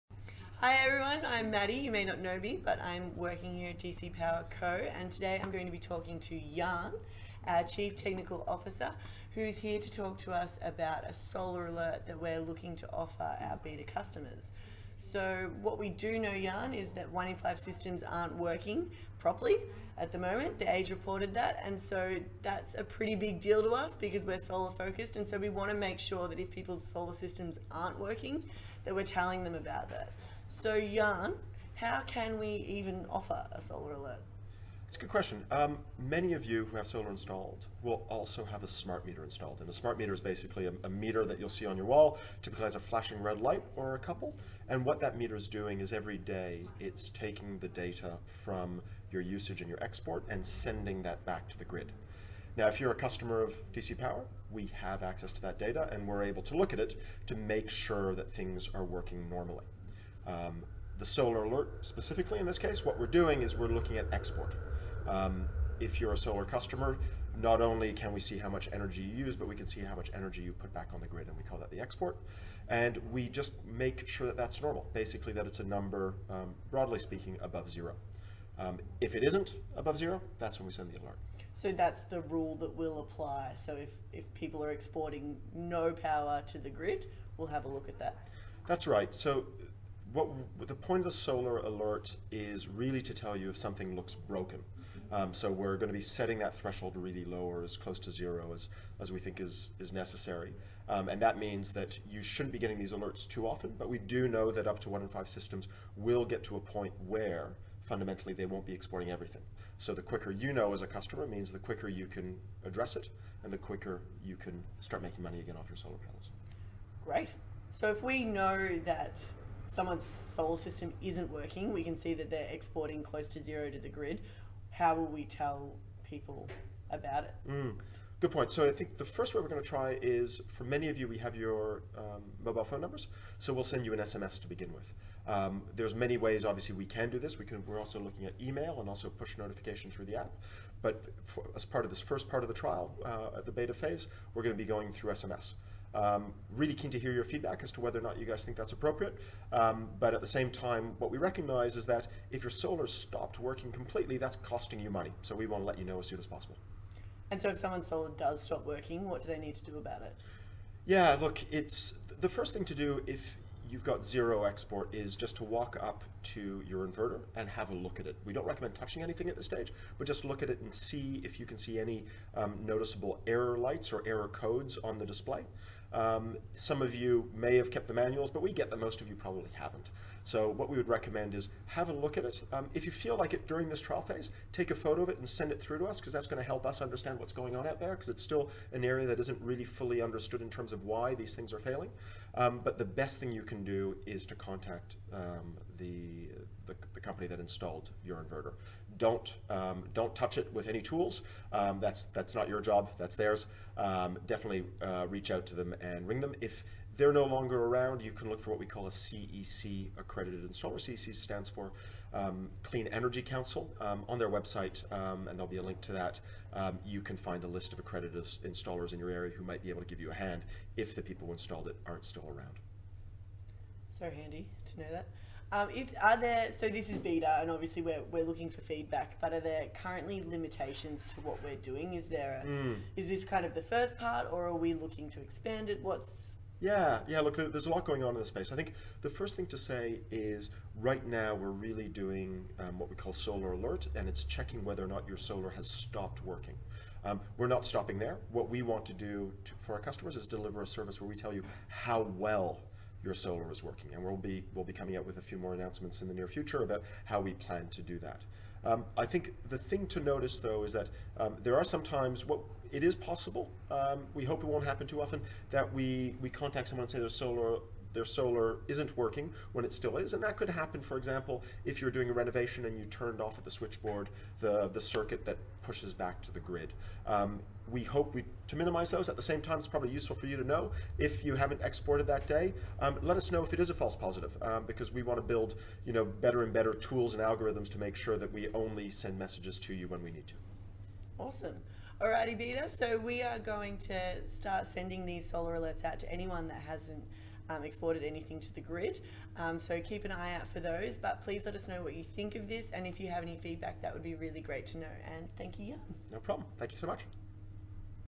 DC Power Co's solar alert: find out if your solar system stops working.
DC Power Co is built by solar people, for solar people. That's why we're developing a solar alert service for our customers, to let you know if your solar system stops working properly.

This week we're joined by DC Power Co CTO Yann Burden to talk through the solar alert and how it will work.